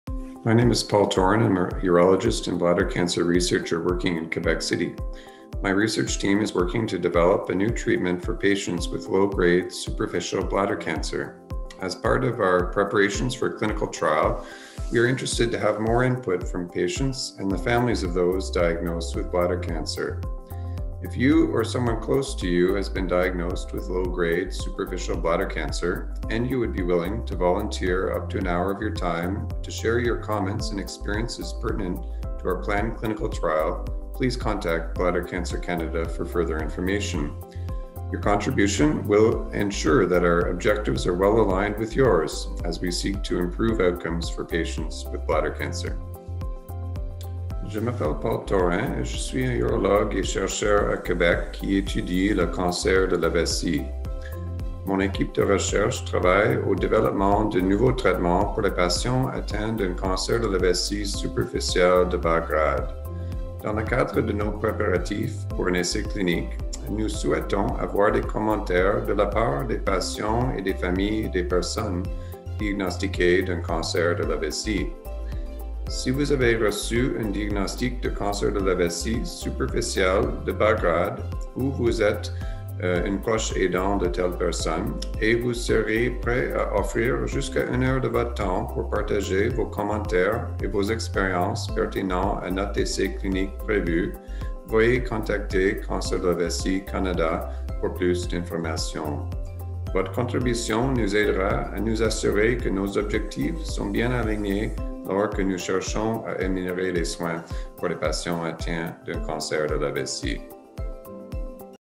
Do you have low grade, non-muscle invasive bladder cancer? Volunteers needed for patient survey!
Dr. Paul Toren, at the University of Laval in Quebec City, is seeking patients and caregivers perspectives leading up to a research project related to treatments for low grade non-muscle invasive bladder cancer patients.

Dr. Toren is looking for patients and or their family members to participate in a 30 minute survey, virtually, to understand patient perspectives.

The research team will follow up with you to better understand your situation and potential participation in the study.